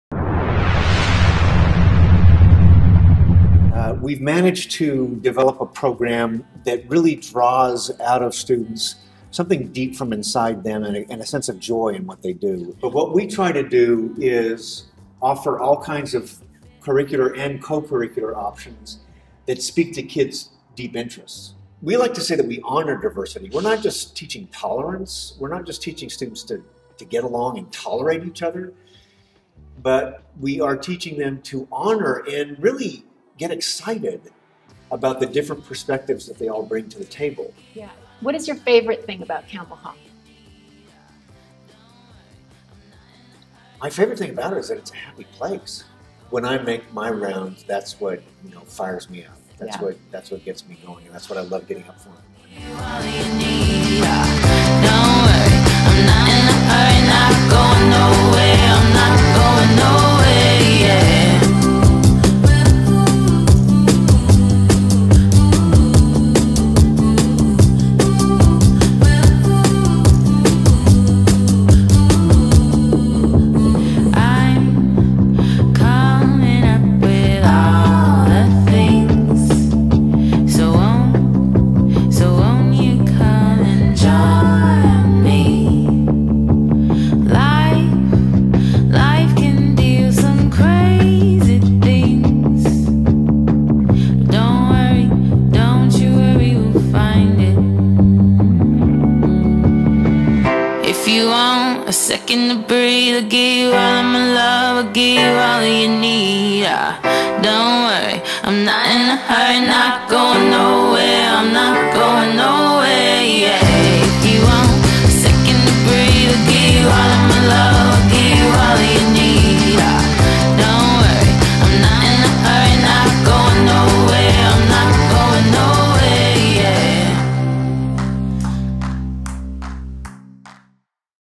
Focus TV Preview | Campbell Hall
Focus TV Host Michelle Gillette tours the campus of Campbell Hall for the upcoming issue of Focus Magazine.